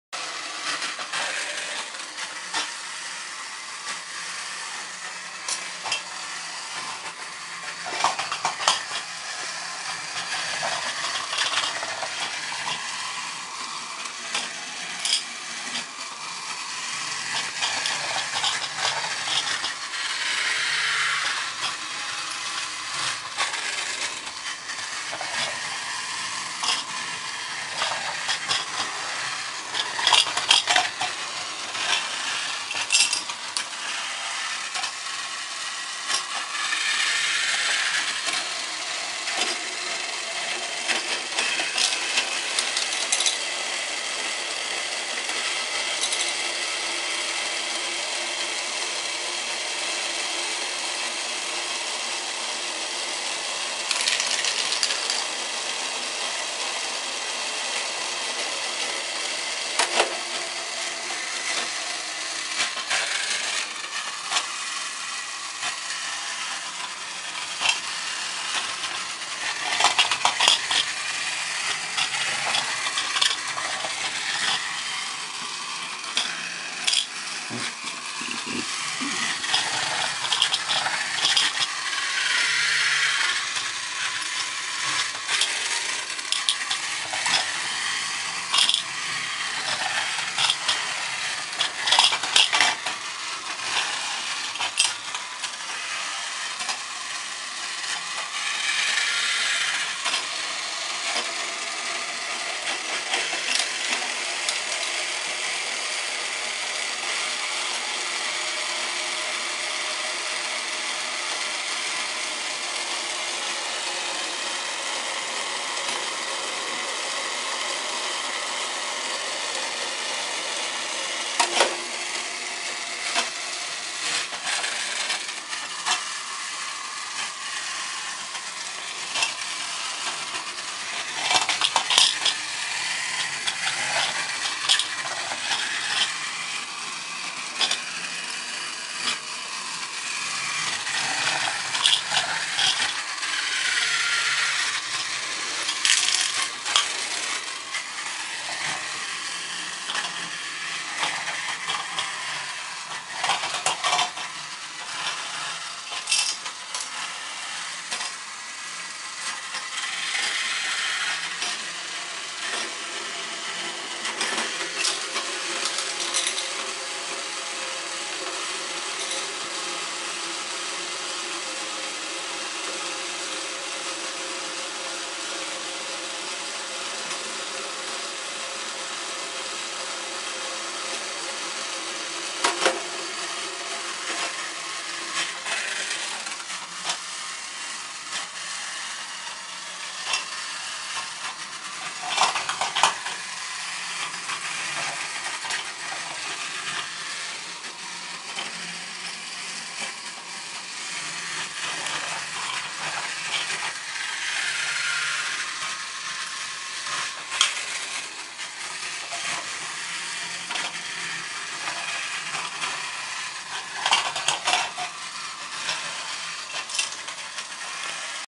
Thomas Postoffice Loader
This video to show how Thomas Postoffice Loader works, which was gifted on the eve of Christmas 2012 to my son. I unpacked and assembled and tried see how it works.